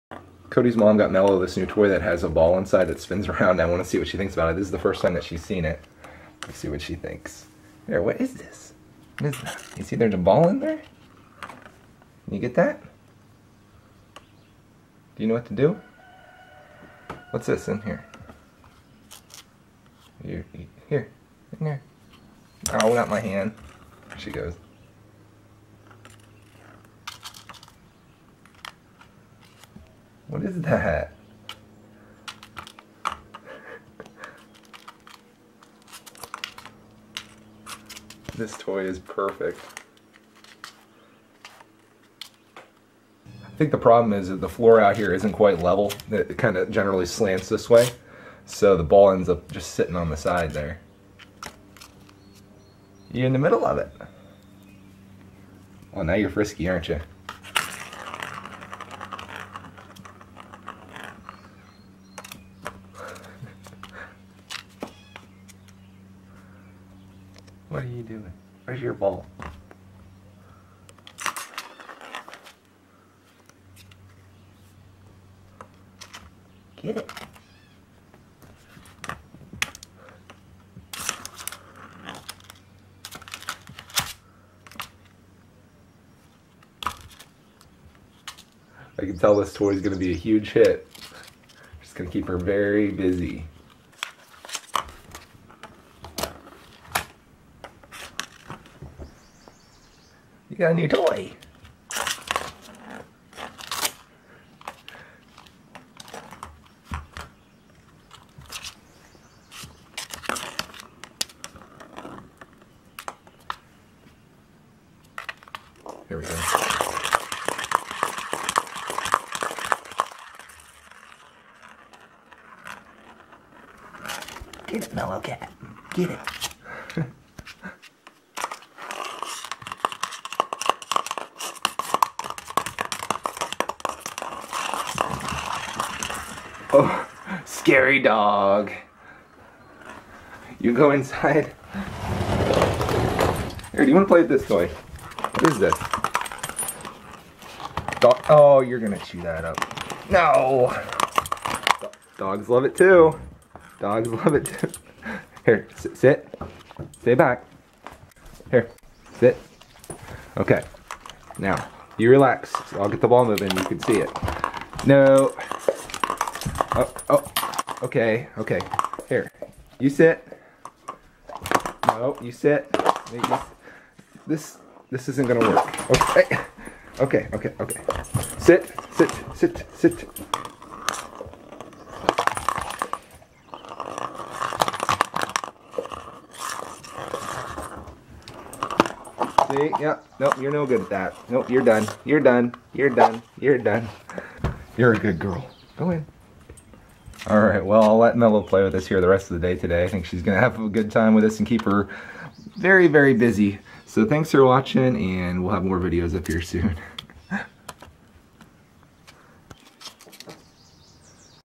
Melo's New Toy
Deb gets Melo a toy but Roxy thinks its for her.